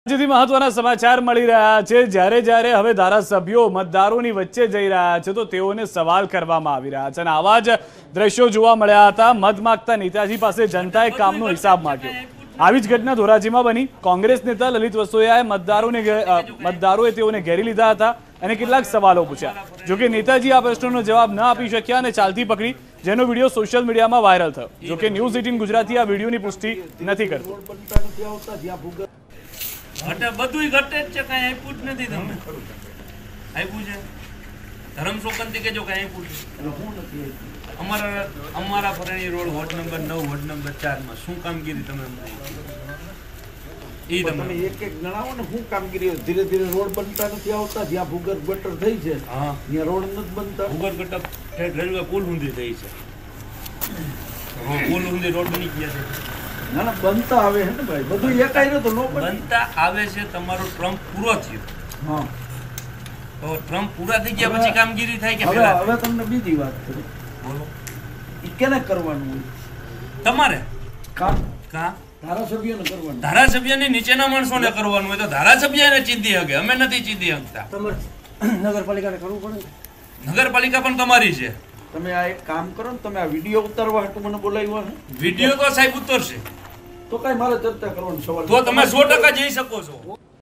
Lalit Vasoya News | મતદારોના સવાલોના જવાબ ન આપી શક્યા નેતાજી | Election 2022 |Congress |Gujarat News
Netaji could not answer the voters' questions.  Incident happened with Lalit Vasoya in Dhoraji.

મતદારોના સવાલોના જવાબ ન આપી શક્યા નેતાજી. Dhoraji માં Lalit Vasoya સાથે થયો બનાવ.n18oc_politics